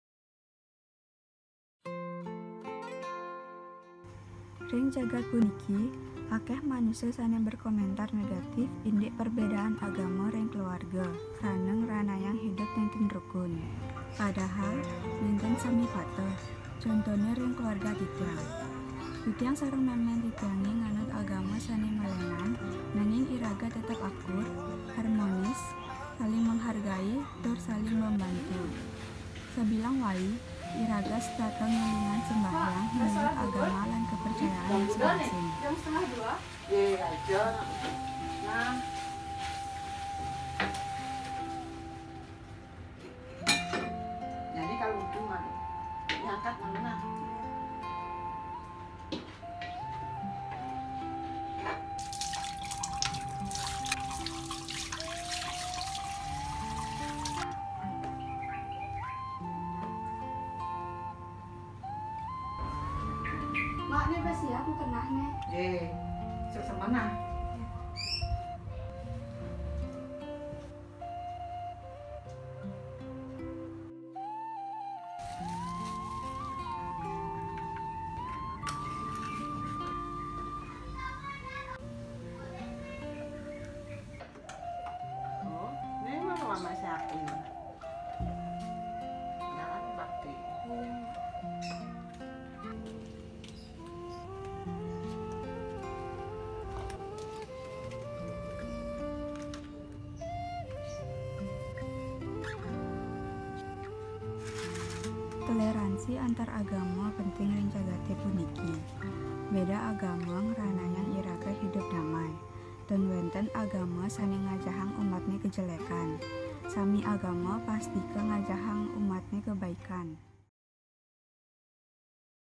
Tolerance between religions in the family
Religious tolerance is very important in this world, religious differences make us live in peace. There is no religion that teaches people badness, all religions must teach goodness.

Produced for Basabali Wiki